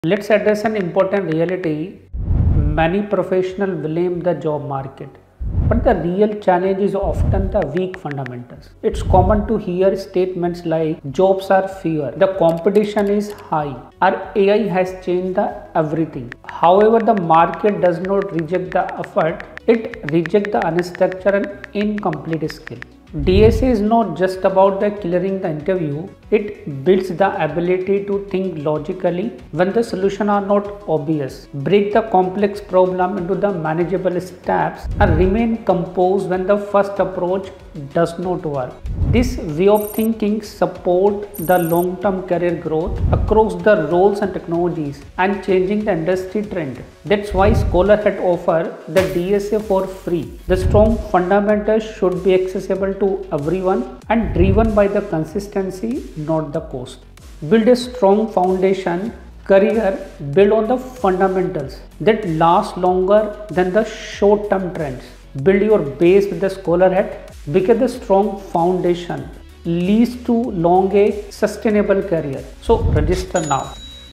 The Real Reason You Aren't Getting Hired (It’s Not AI)
Why we made DSA Free (The Reality Check) 💡

"The market rejects unstructured and incomplete skills."

In this video, we break down why relying on short-term trends is dangerous for your career. DSA (Data Structures and Algorithms) is the bedrock of logical thinking. It teaches you how to remain composed when code breaks and how to find solutions when they aren't obvious.

We believe education should be driven by consistency, not cost.

That’s why ScholarHat has offered DSA for FREE.